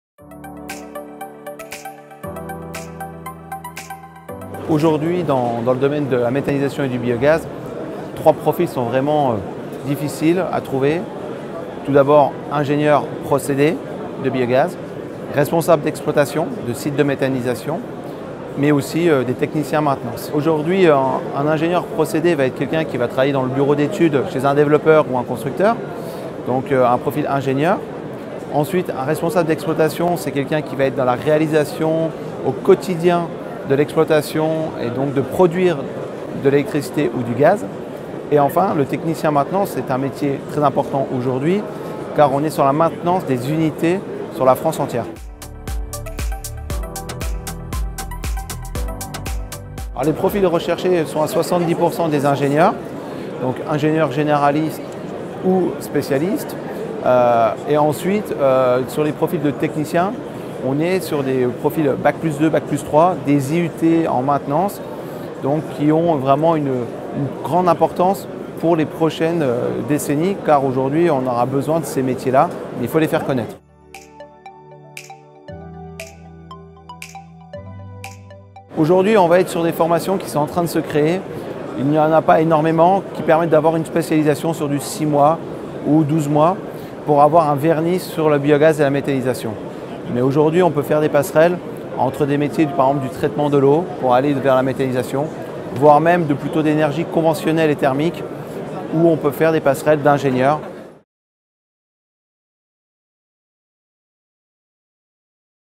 JP Burtin (Borea) : "Les profils recherchés dans le biogaz sont à 70 % des ingénieurs"
Interview de Jean-Philippe Burtin, directeur commercial du cabinet de recrutement Borea recrutement, à l'occasion du salon Biogaz Europe, à Rennes, le 30 janvier 2019.